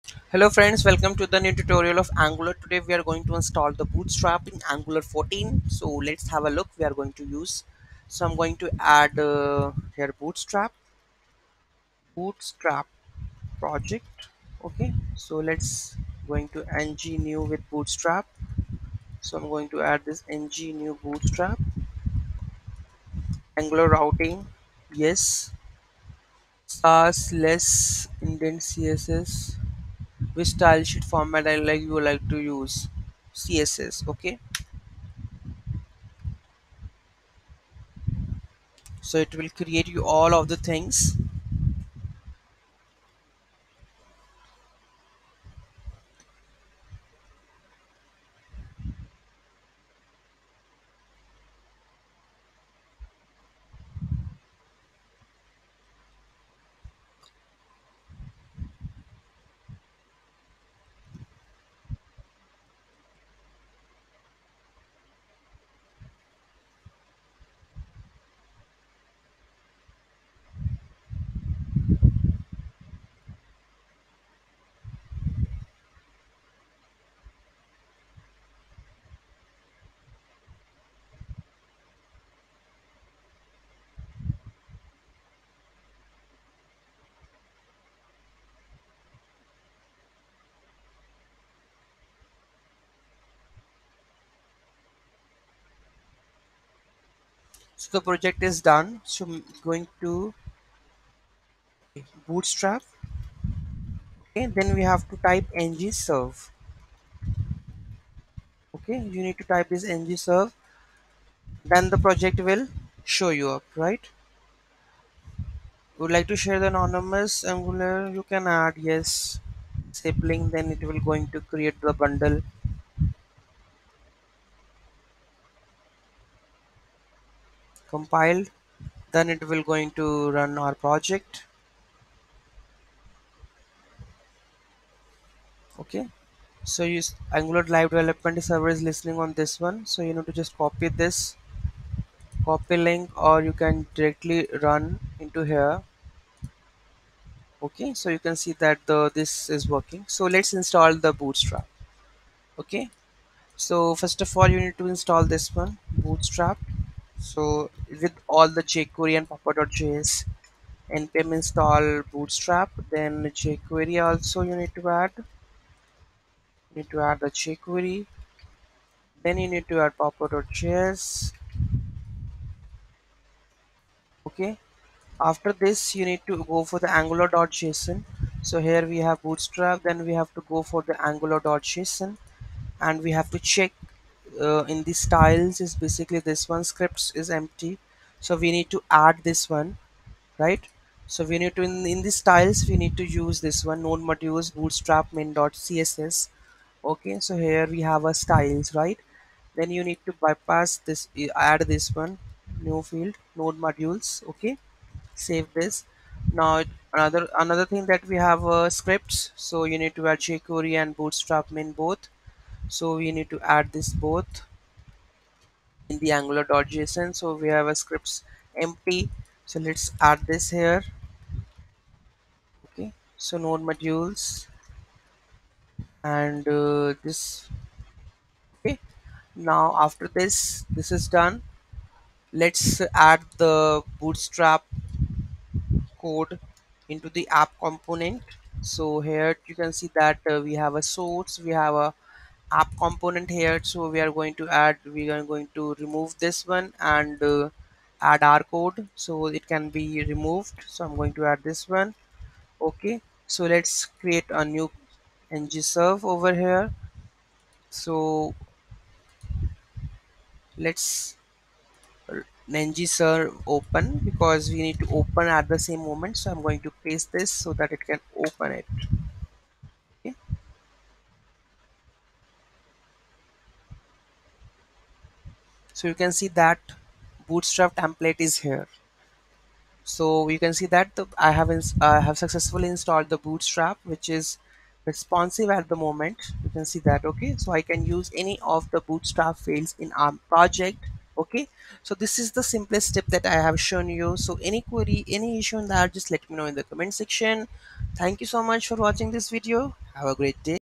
How to Add Bootstrap to Angular 14
How to Add Bootstrap to Angular

npm install bootstrap --save
npm install jquery --save
npm install popper.js --save
      "styles": [

        "node_modules/bootstrap/dist/css/bootstrap.min.css",

        "src/styles.css"

      "scripts": [

          "node_modules/jquery/dist/jquery.min.js",

          "node_modules/bootstrap/dist/js/bootstrap.min.js"